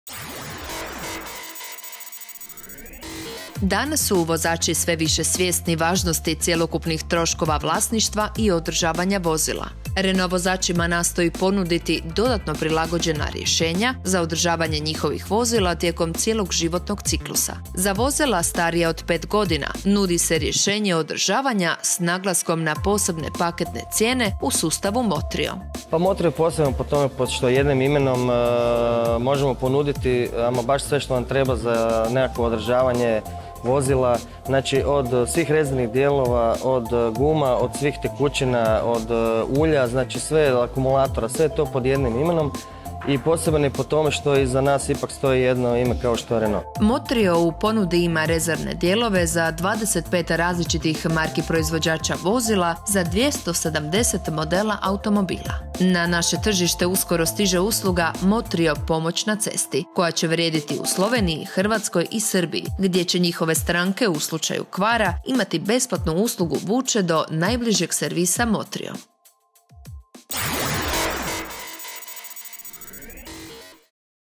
Motrio - rješenje za održavanje vozila
Održavanje vozila starijih od 5 godina uz posebne paketne cijene nudi Motrio. :)